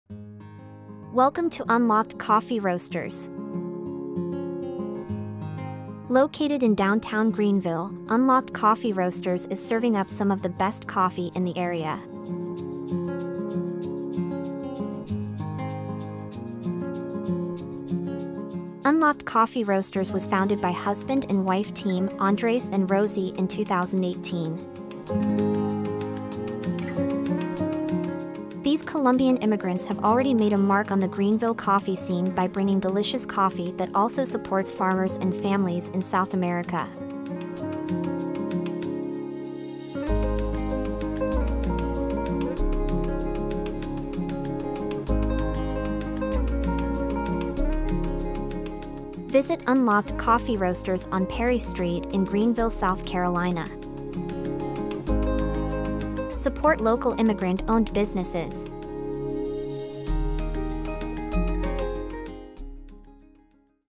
Supporting Local Immigrant Owned Businesses: Unlocked Coffee Roasters
Located in downtown Greenville, South Carolina Unlocked Coffee Roasters is serving up some of the best coffee in the area.

Unlocked Coffee Roasters was founded by husband and wife team Andres and Rosie in 2018. These Columbian immigrants have already made a mark on the Greenville coffee scene by bringing delicious that also supports farmers and families in South America.

Immigrant owned local businesses like Unlocked Coffee Roasters are making the United States a better place.

Do you have a dream of starting your own business in the United States? Learn more about the immigration process and beginning your green card journey today with BDV Solutions